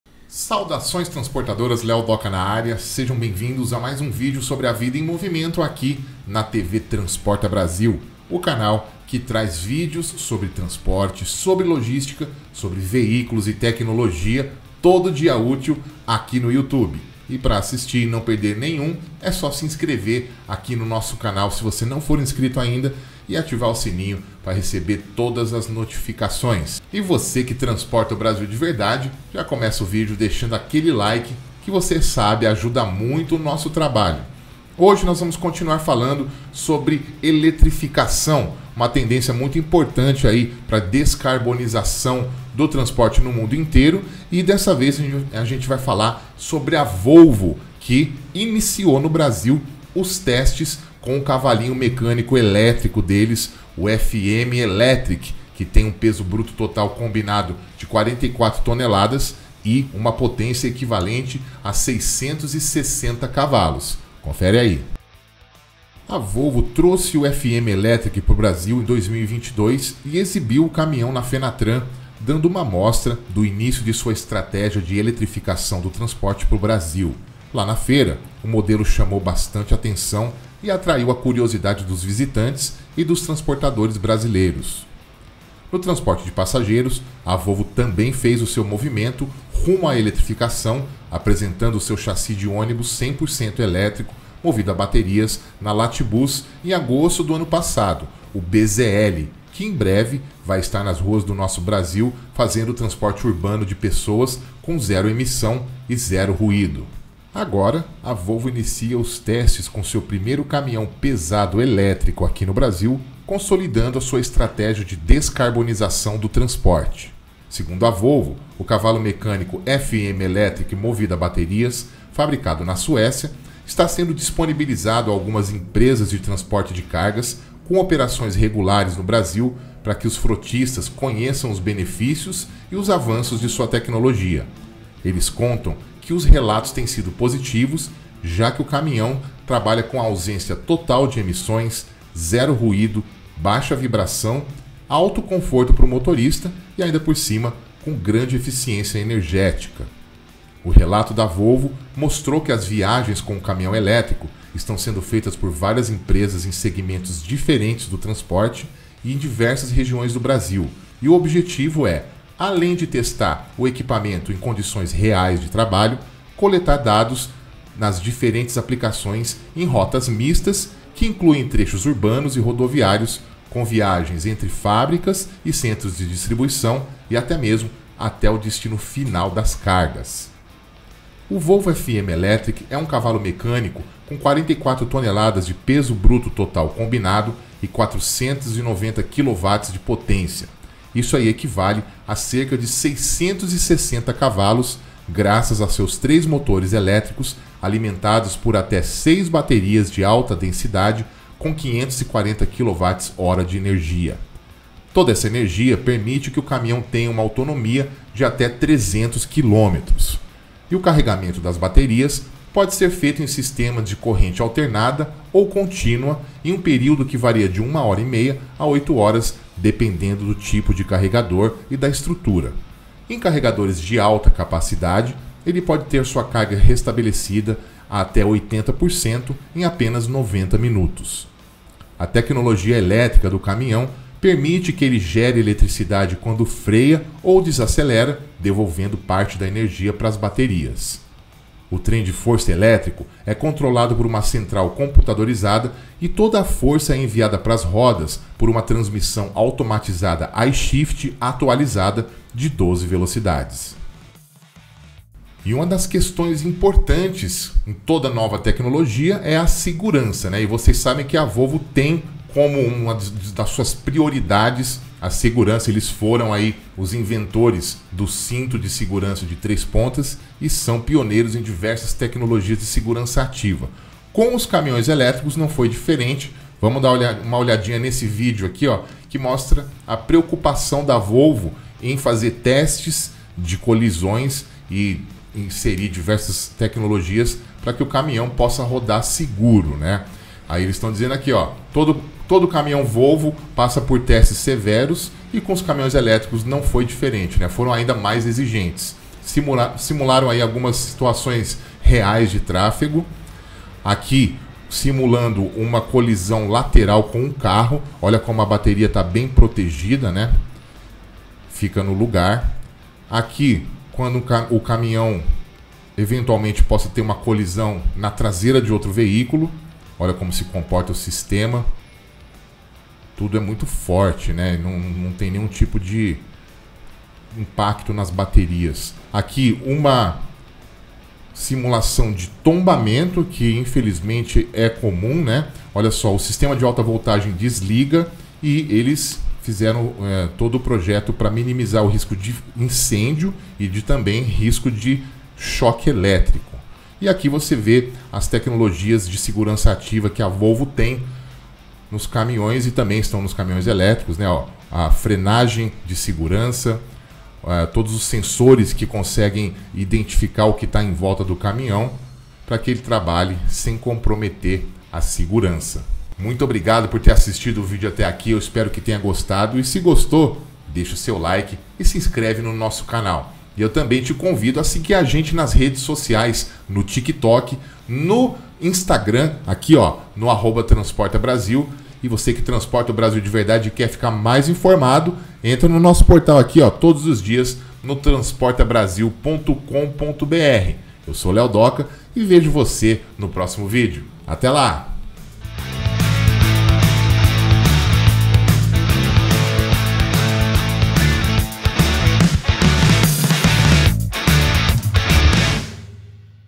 VOLVO TESTA CAMINHÃO FM ELÉTRICO NO BRASIL
Clientes brasileiros de transportadoras de diversos segmentos já estão testando o FM Electric, cavalo mecânico pesado elétrico movido a baterias com 44 toneladas de Peso Bruto Total Combinal e autonomia de até 300 quilômetros.